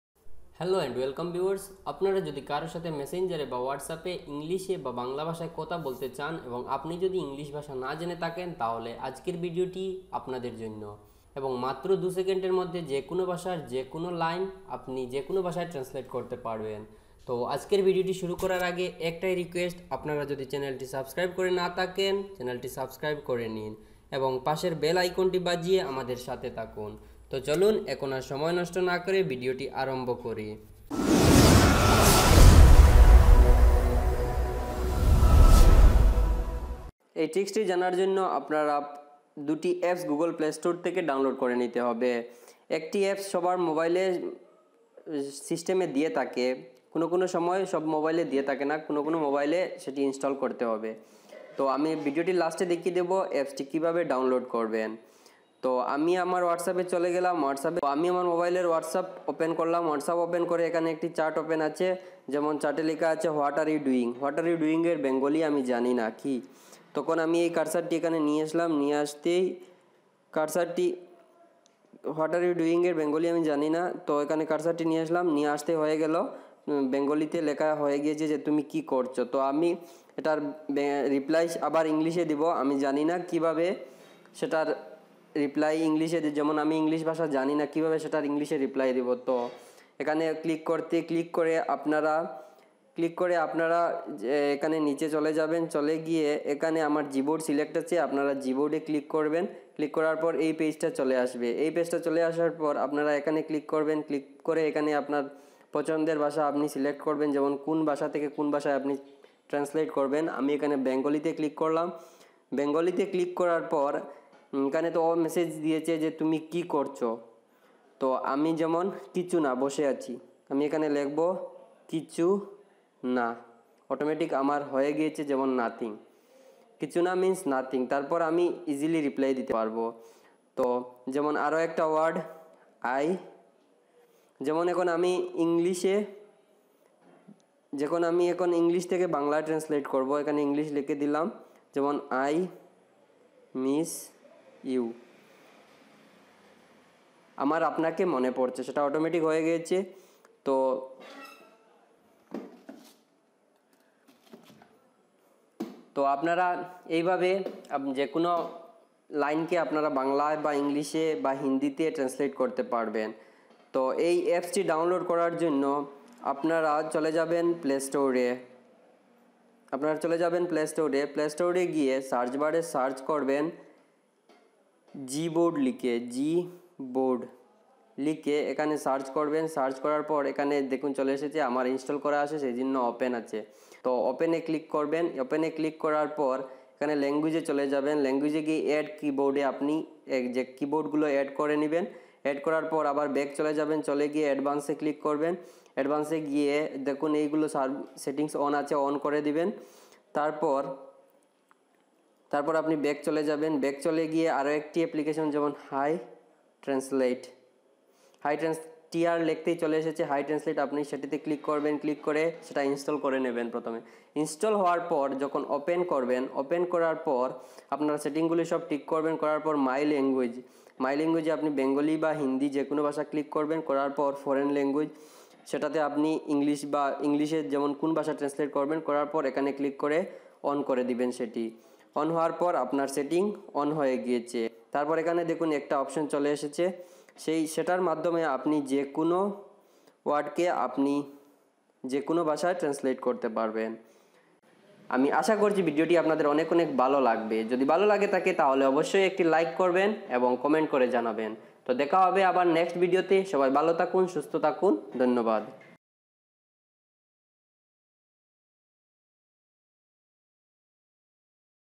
বাংলা লিখুন ইংলিশ হয়ে যাবে অধিকাংশ লোকে জানে না || Bengali to English Translate
Dear Friends ajker video ti te ami apnader k dekhabo j ki babe matro 1 second a j kuno basha tek a j kuno basay translate kora abong ki babe apni apnar mobile keyboard diye bengali type korben.Apni j kuno basay translate kor a j karo sathe j kuno basay chating korte parben.asha kori video ti apnader onek onek valo lagbe.
মাত্র ১ মিনিটে নিজের নাম দিয়ে DJ গান তৈরি করুন || How to Make DJ Song of your Own Name||